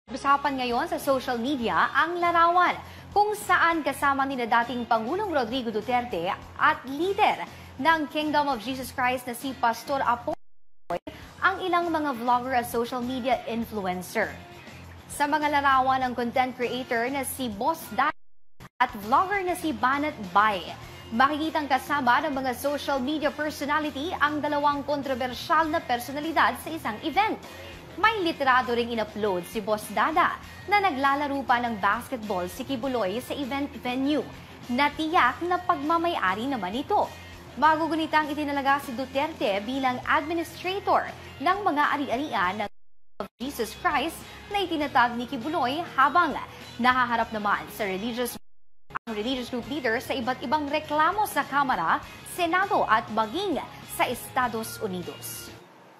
DIGONG, QUIBOLOY, MAY PASABOG NGA BA MATAPOS MAISPATAN KASAMA MGA VLOGGER?
Usap-usapan ang larawan ni Dating Pangulong Rodrigo Duterte at KOJC Leader Pastor Apollo Quiboloy kasama ang ilang vlogger at content creator.